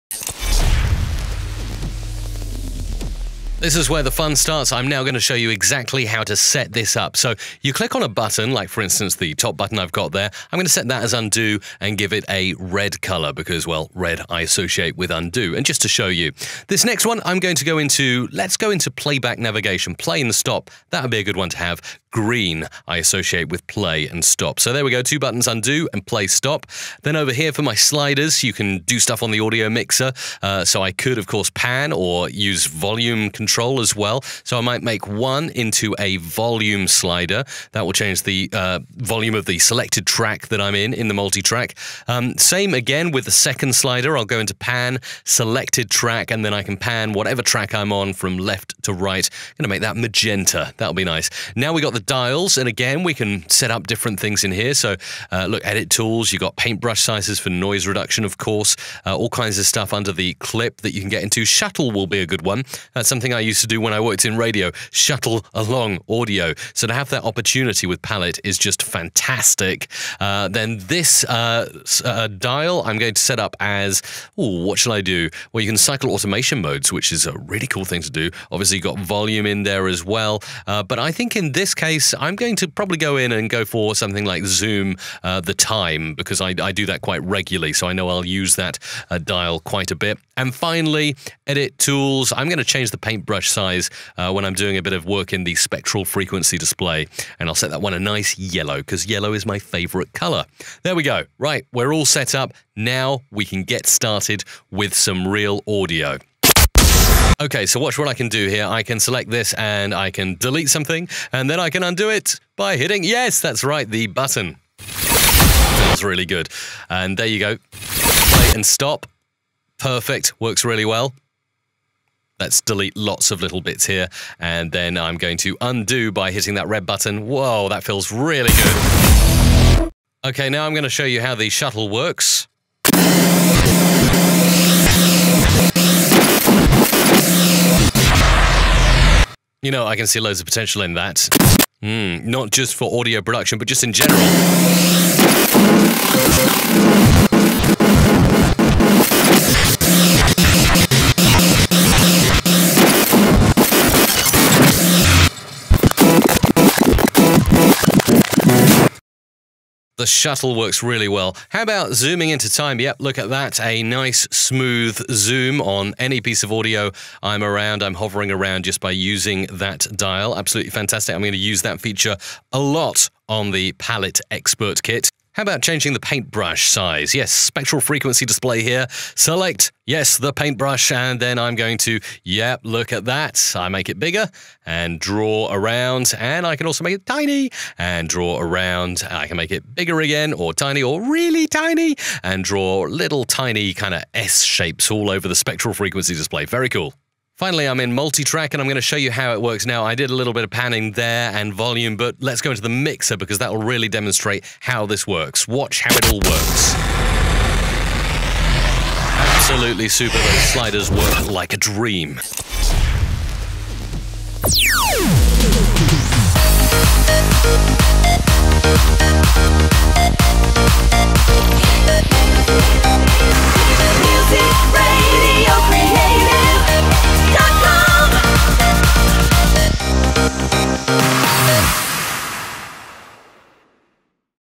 Palette Gear Music and Audio Editing Examples
Here's how to make Palette Gear rock with Adobe Audition. You can perform so many audio editing tasks with ease using the buttons, dials and sliders of Palette Gear!

0:00 Tutorial start
0:01 Setting up
1:46 Using in Audition